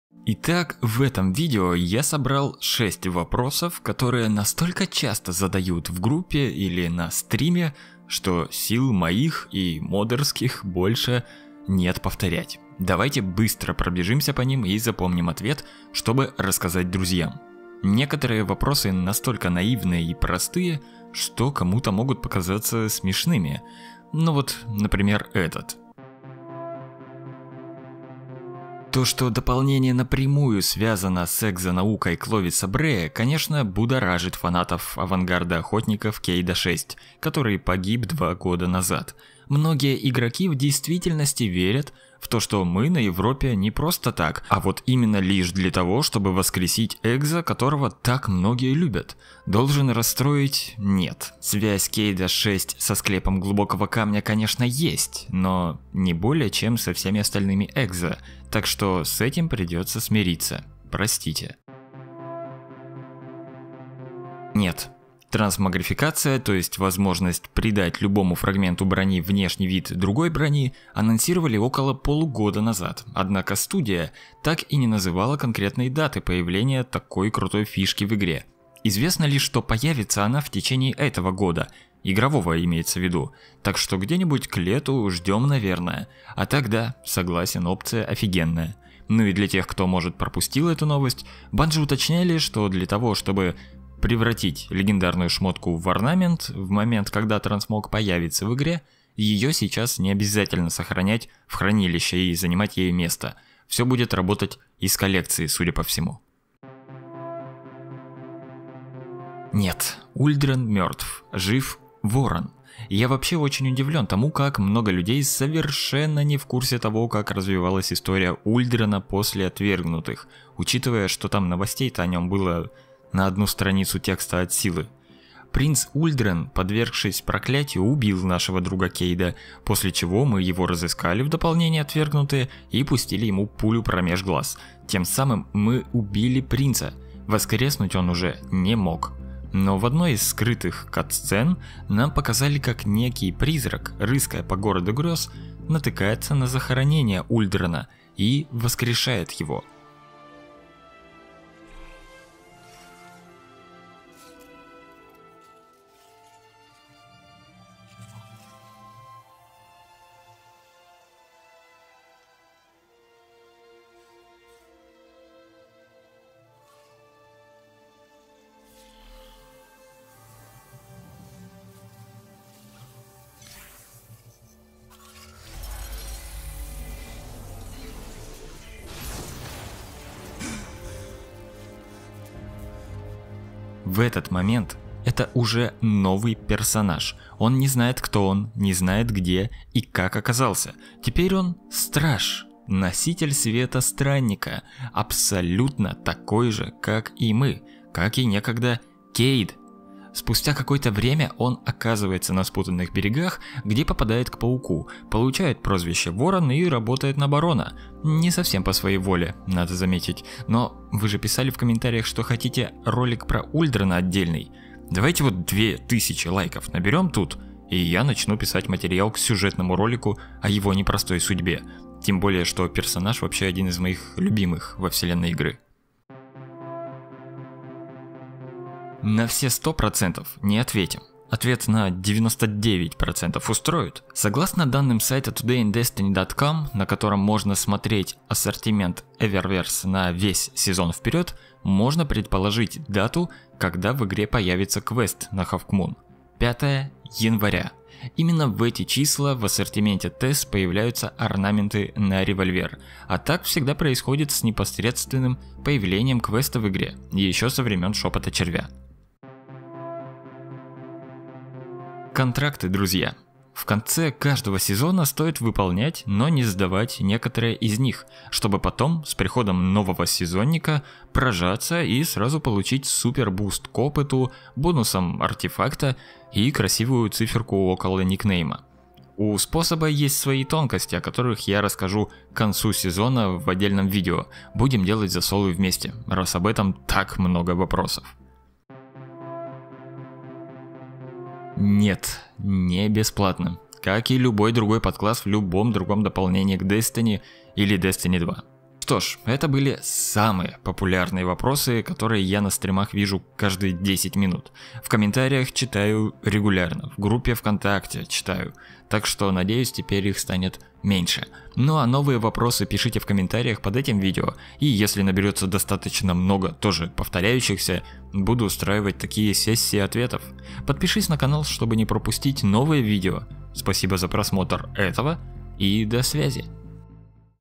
Destiny 2. Шесть популярных вопросов. Хавкмун. Ульдрен. Кейд. Трансмог...
А когда Хавкмун? А почему Ульдрен жив? А где трансмог? А мы воскресим Кейда на Европе?. ох как часто я вижу эти вопросы у себя на стриме.

Смотри мои ролики до их релиза на канале на странице Boosty!